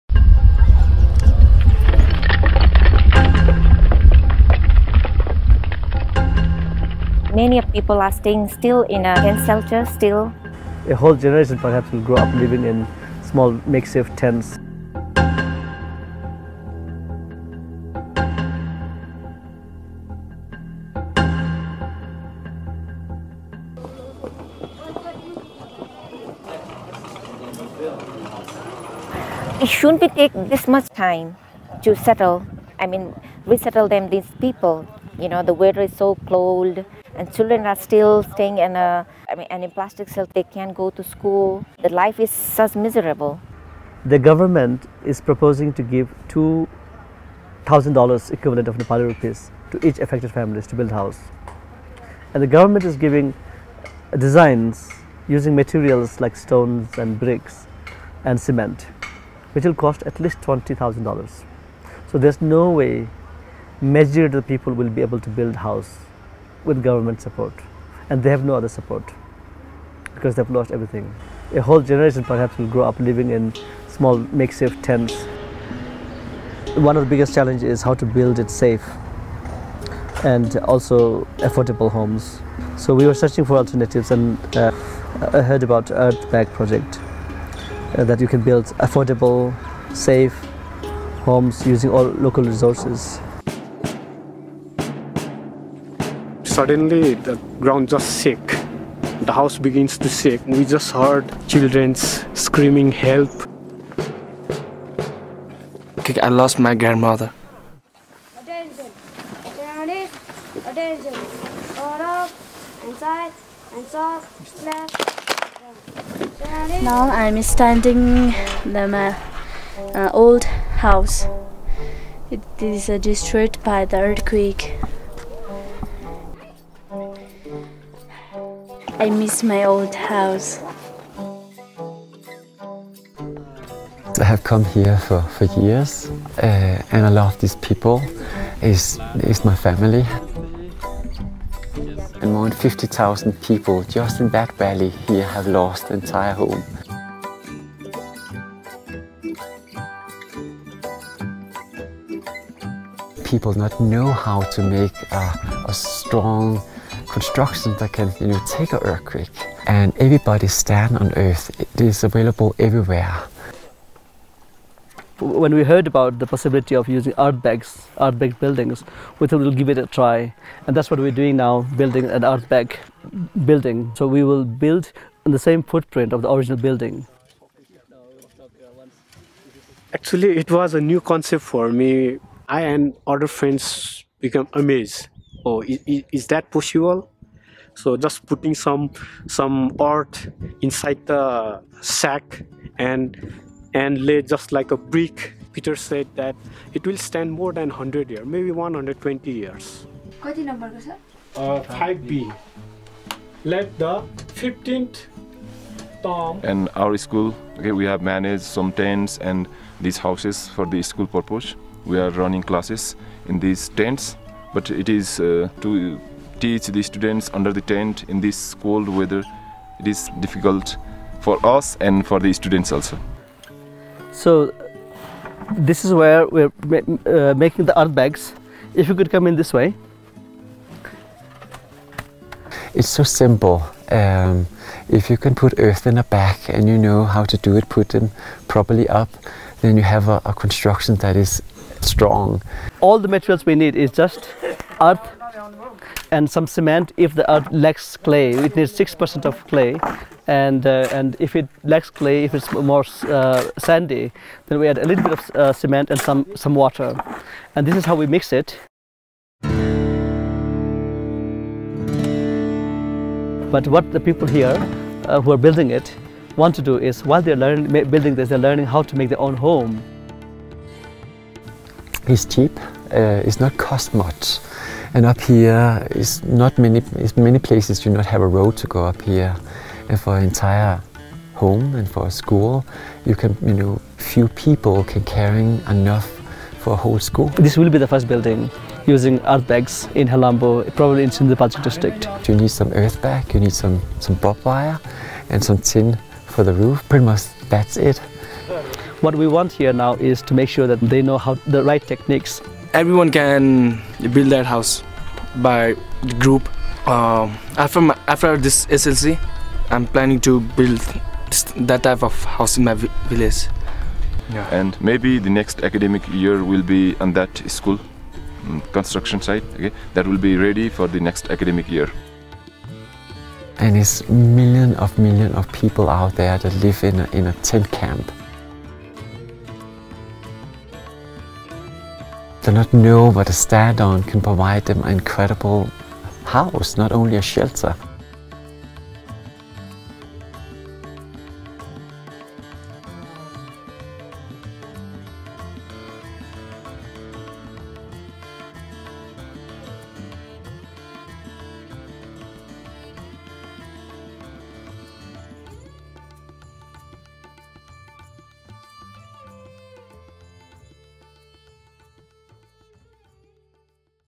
Nepal Earthquake Victims Build Earthbag Homes
Earthquake recovery is taking much longer than expected since the 2015 quake in Nepal. Thousands of families still live in temporary tent shelters. In this video Peter Jensen from motherearthbuildings.com and Gopal Lama from the Just Nepal Foundation explain and explore the incredible promise of 'earthbag buildings'. Earthbag construction is a technique made popular by the late Iranian architect Nader Khalili. It is cheap, sustainable and could help in many regions around the world where individuals are stuck living in temporary shelters.